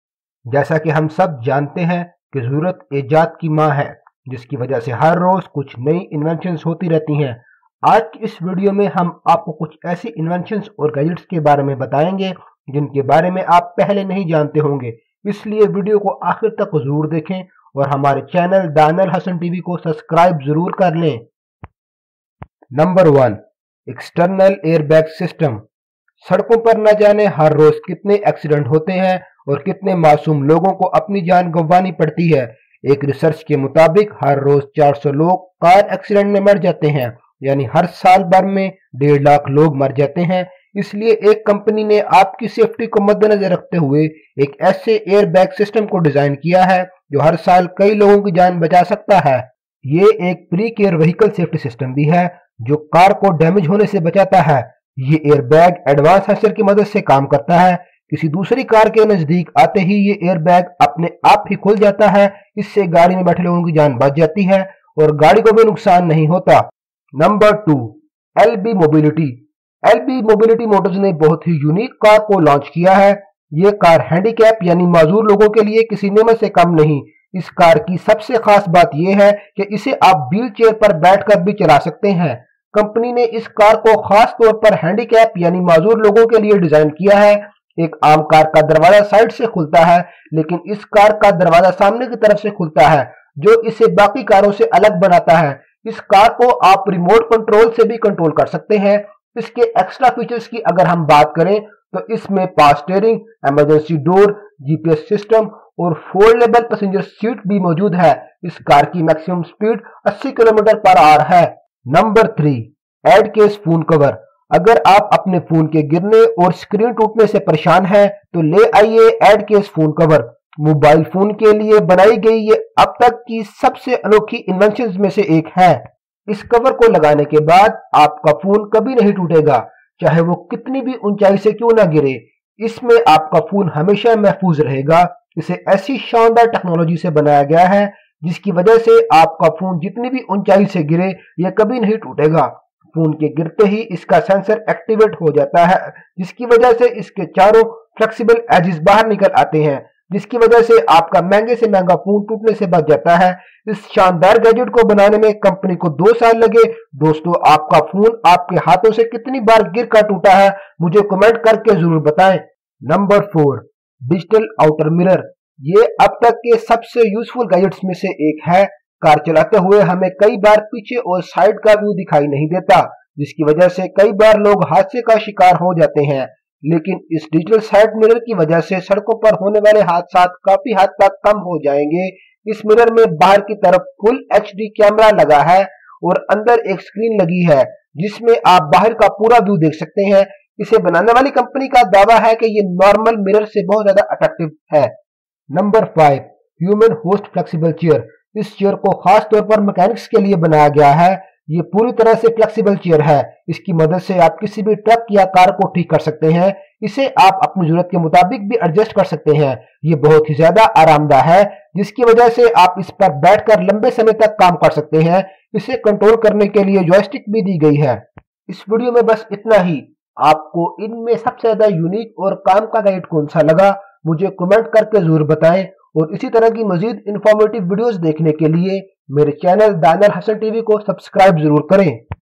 5 Advance Technology Inventions and Gadgets that can change your life Presented by Daanial Hassan Tv
5 Advance Technology Inventions and Gadgets that can change your life Presented by Daanial Hassan Tv.
Here is the list of top 10 Technology that will change your life -

1) External Airbag System

2) Elbee Mobility

3) Ad Case phone cover

4) Digital Outer Mirror

5) Human Host flexible chair